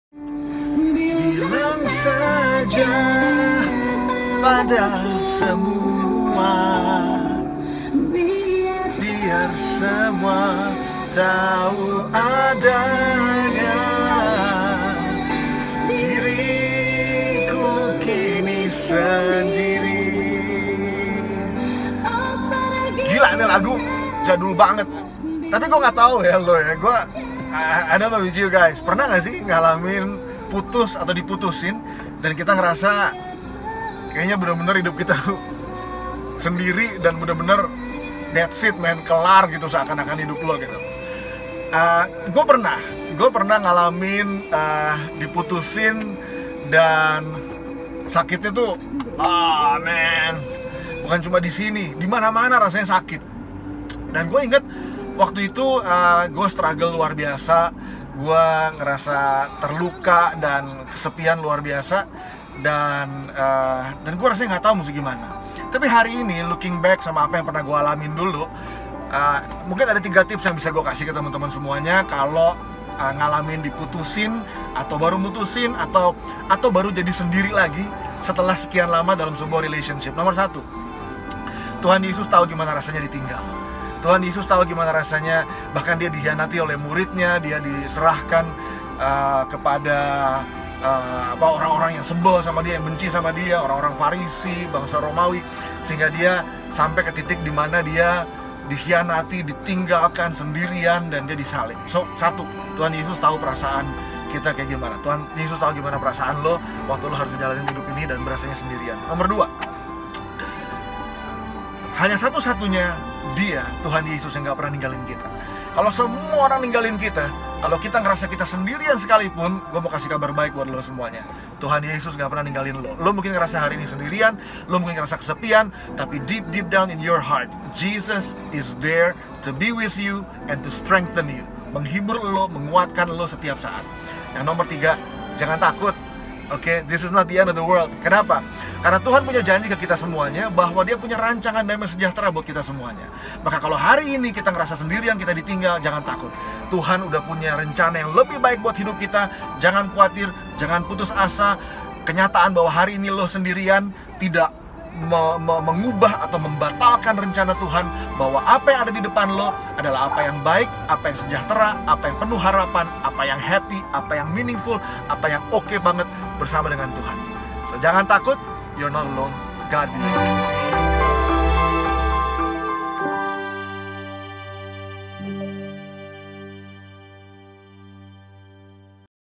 INSPIRE VLOG #47 | "3 Tips Abis Diputusin" | Riko Ariefano
Pernah nggak sih ngerasain diputusin, yes, sakitnya di mana-mana. Kuy simak 3 tips dari Riko Ariefano.

INSPIRE YOUNG PEOPLE
Make better decisions in Christ